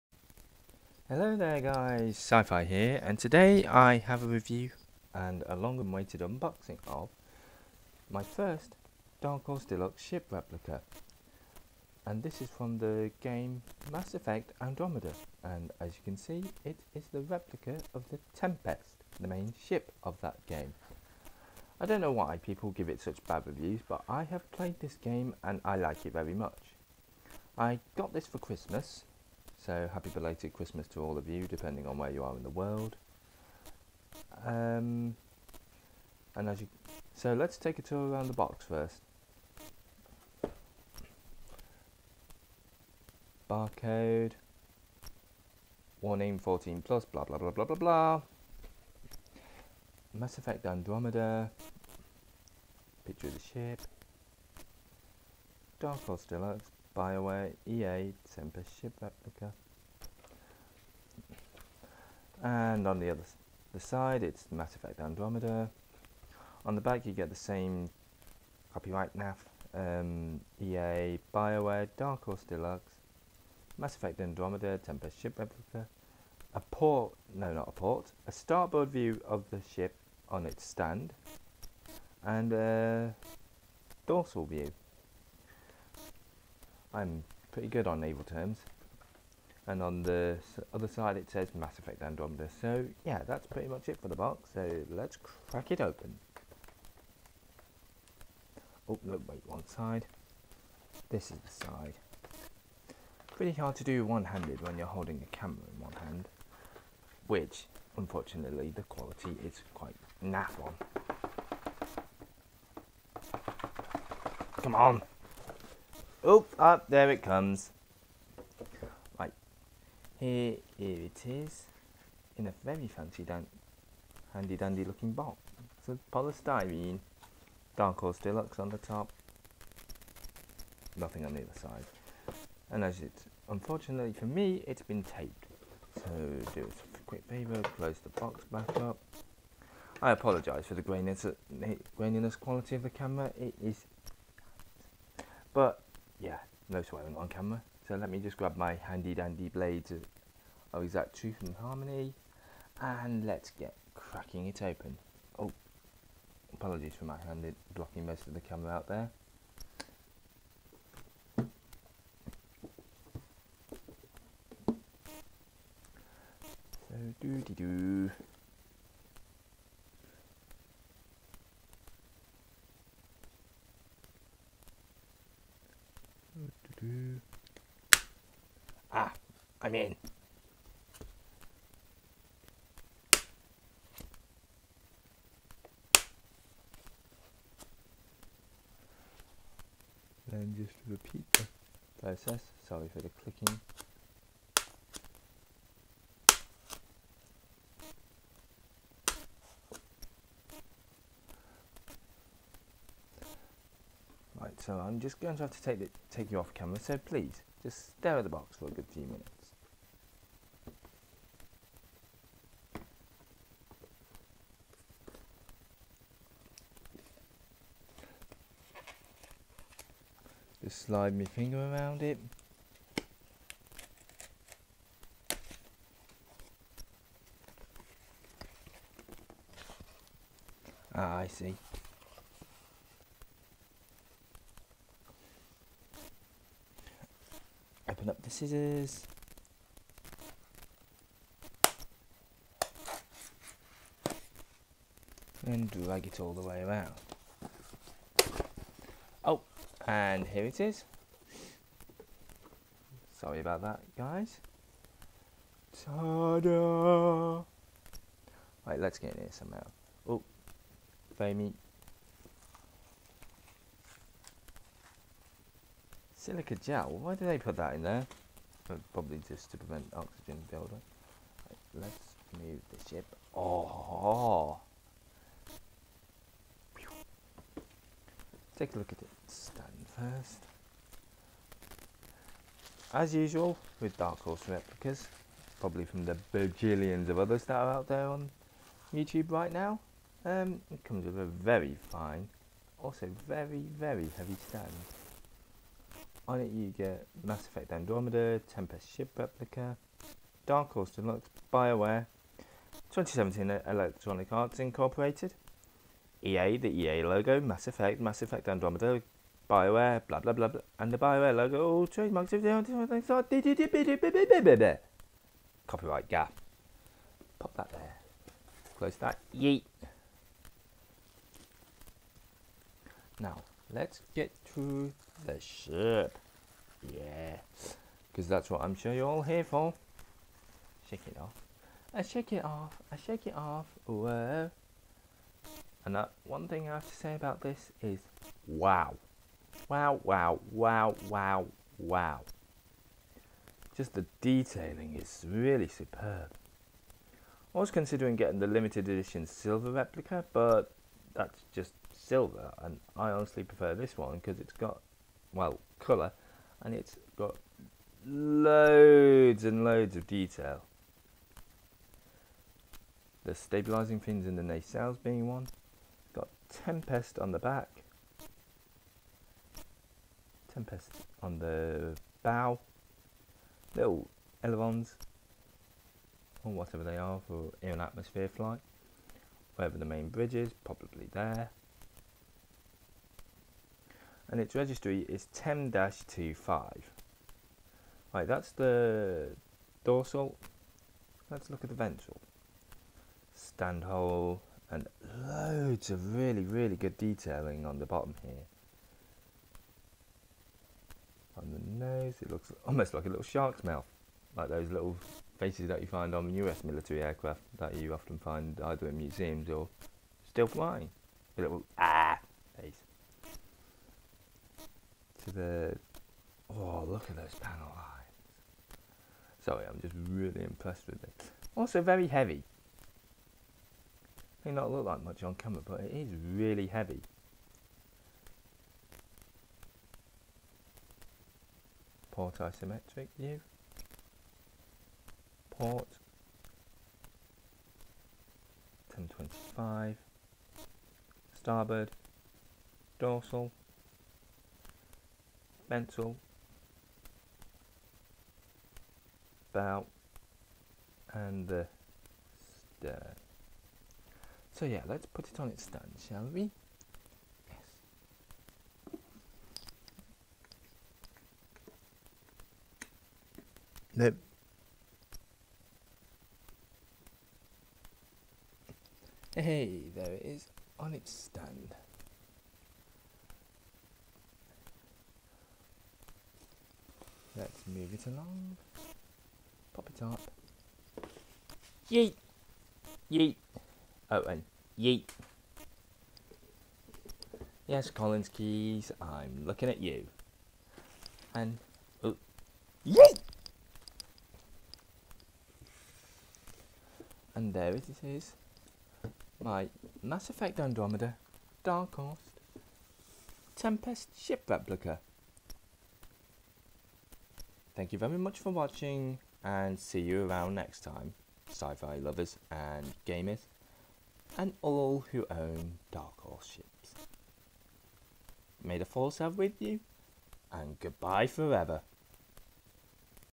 Dark Horse Mass Effect Andromeda Tempest Ship replica Unboxing and Review
A Replica of the Tempest from Mass Effect Andromeda. Likely the first review of it on Youtube! Enjoy! Edit : Thanks for making this my most popular video folks!
PS. Audio is terrible for unknown reasons. If you want to mute it, go ahead.